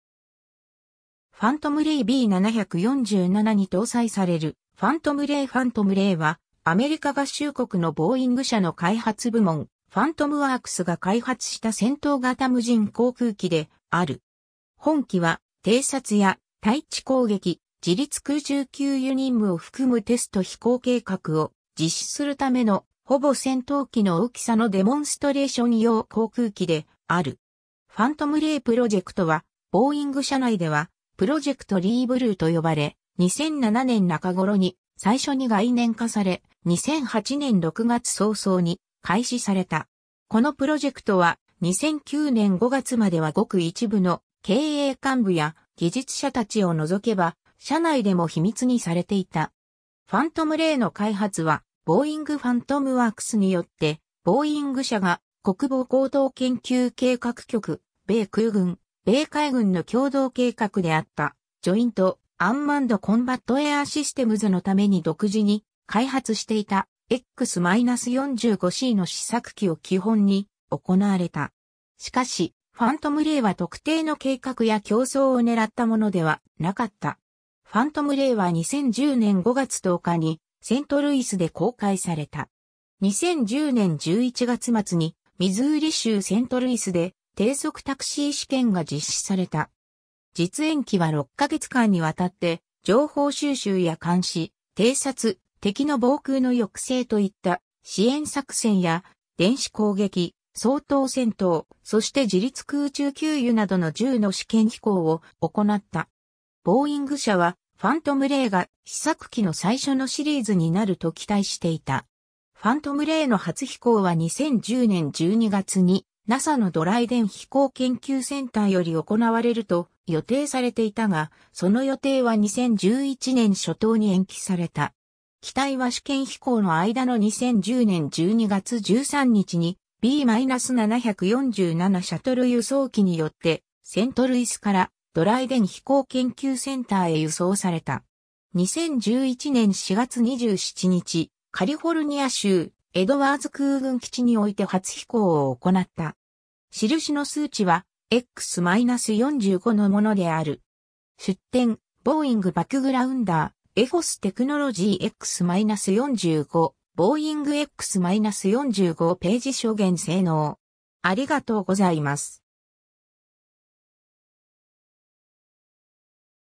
ファントム・レイ
Phantom Ray B747 (N905NA)に搭載されるファントム・レイ ファントム・レイ (Phantom Ray) は、アメリカ合衆国のボーイング社の開発部門ファントムワークスが開発した戦闘型無人航空機 (UCAV, unmanned combat air vehicle, uninhabited combat aerial vehicle) である。
本機は偵察や、対地攻撃、自律空中給油任務を含むテスト飛行計画を実施するための、ほぼ戦闘機の大きさのデモンストレーション用航空機である。
 「ファントム・レイ」プロジェクト (The Phantom Ray project) は、ボーイング社内では "Project Reblue" と呼ばれ、2007年中頃に最初に概念化され、2008年6月早々に開始された。
このプロジェクトは、2009年5月まではごく一部の経営幹部や技術者達を除けば社内でも秘密にされていた。
 ファントム・レイの開発はボーイング ファントムワークスによって、ボーイング社が国防高等研究計画局 (DARPA, Defense Advanced Research Projects Agency)/米空軍/米海軍の共同計画であった"Joint-Unmanned Combat Air Systems"（J-UCAS、統合無人戦闘航空システム）のために独自に開発していたX-45Cの試作機を基本に行われた。
しかし、ファントム・レイは特定の計画や競争を狙ったものではなかった。
 ファントム・レイは2010年5月10日にセントルイスで公開された。
2010年11月末にミズーリ州セントルイスで低速タクシー試験が実施された。
実演機は6ヶ月間にわたって、情報収集や監視、偵察、敵の防空の抑制といった支援作戦や、電子攻撃 (electronic attack)、掃討戦闘、そして自律空中給油などの10の試験飛行を行った。
ボーイング社は、ファントム・レイが試作機の最初のシリーズになると期待していた。
ファントム・レイの初飛行は2010年12月にNASAのドライデン飛行研究センターより行われると予定されていたが、その予定は2011年初頭に延期された。
 機体は試験飛行の間の2010年12月13日にB-747シャトル輸送機 (N905NA) によってセントルイスからドライデン飛行研究センターへ輸送された。
2011年4月27日、カリフォルニア州エドワーズ空軍基地において初飛行を行った。
 "*"印の数値はX-45のものである。
 出典:  Boeing backgrounder, Airforce Technology X-45, Boeing X-45 page 諸元 性能